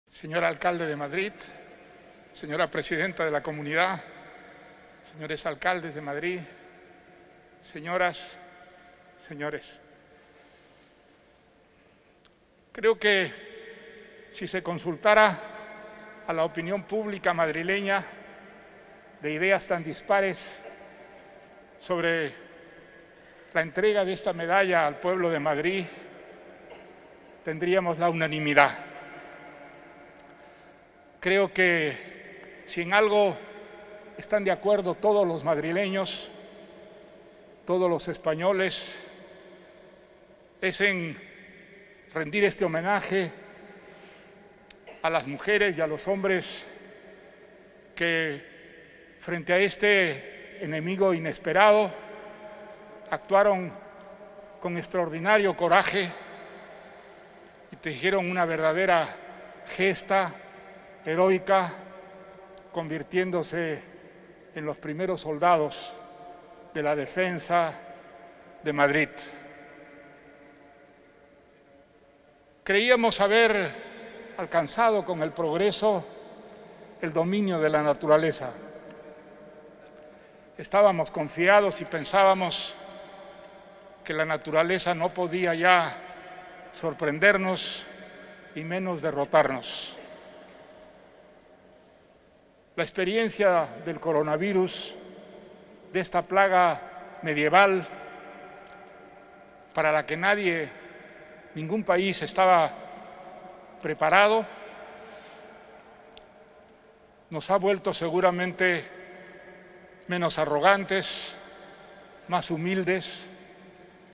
Vargas Llosa alaba a los madrileños, "soldados" que han luchado contra el Covid-19
El escritor premio Nobel de Literatura, Mario Vargas Llosa, ha alabado la acción del pueblo de Madrid frente al Covid-19, a quienes ha descrito como unos "soldados" que han luchado "a manos descubiertas" contra esta "pandemia medieval" que ha adoptado en ocasiones forma de "demonios".